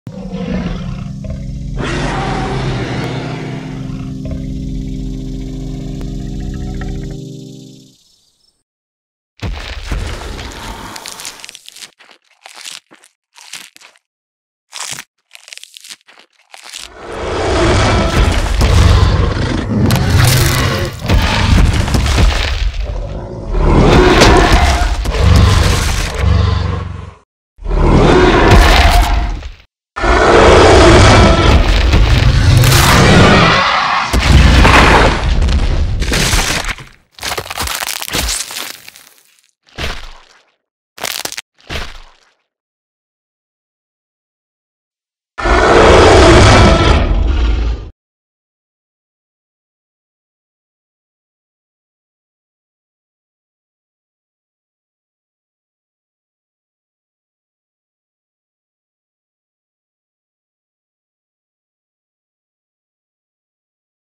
Therizinosaurus vs Tarbosaurus | Clay dinosaur fight
You can request your own fights in the comments.

Thanks to AlexTriceratops123, AsylusGoji91 Studios and alch 4654 for the sound effects.